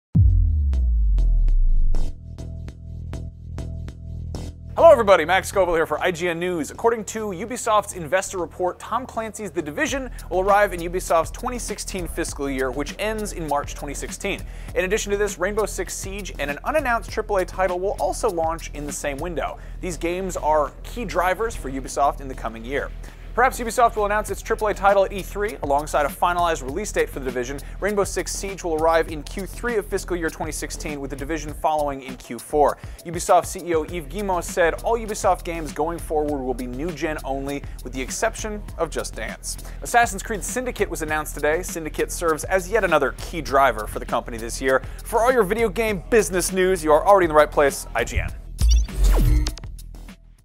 The Division Delayed to 2016 - IGN News
Tom Clancy's The Division will arrive in Ubisoft's 2016 fiscal year, which ends in March 2016. In addition to this, Rainbow Six Siege and an unannounced AAA title will also launch in the same window.